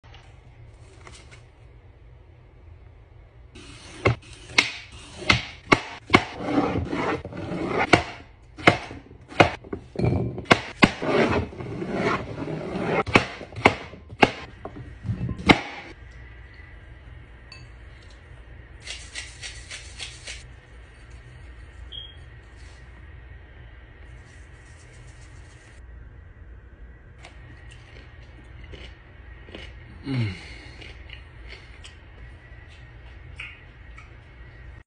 Cucumber and Tajin Summer Snack Fresh From the Garden
Making Cucumber and Tajin Summer Snack fresh From the Garden.